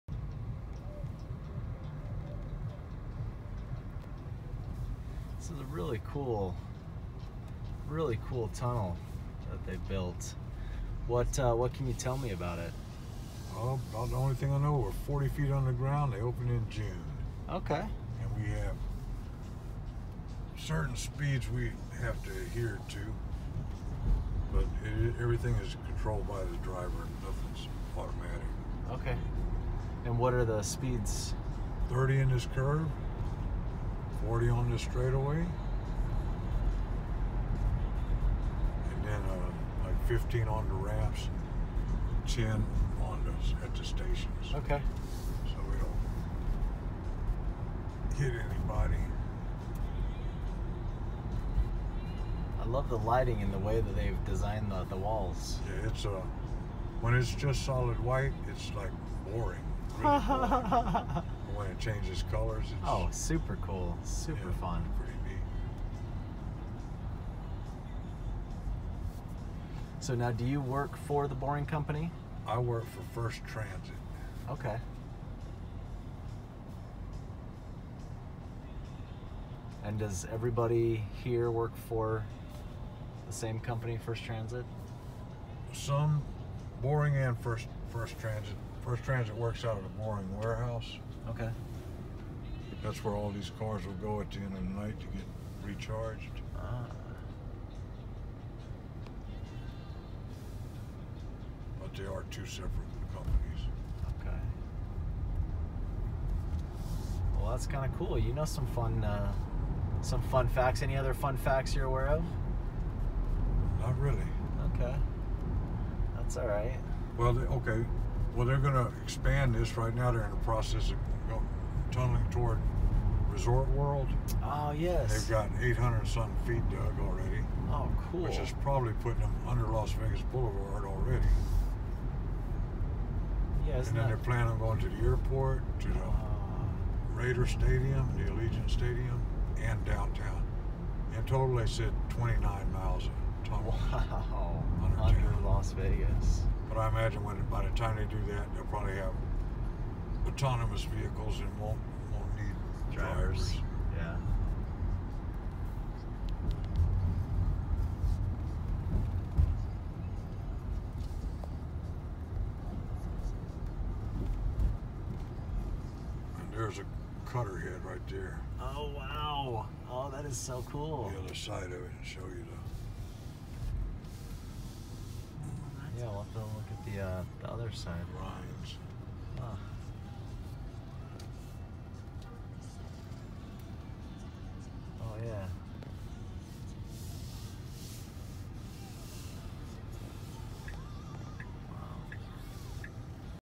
Elon Musk's Tunnel under Vegas!
What a trip go through these lit-up tunnels that The Boring Company built. Naturally the only vehicles allowed were Tesla's and we discovered that they only turn the lights on inside the tunnels for special occasions like the SEMA convention. Which is why we were there.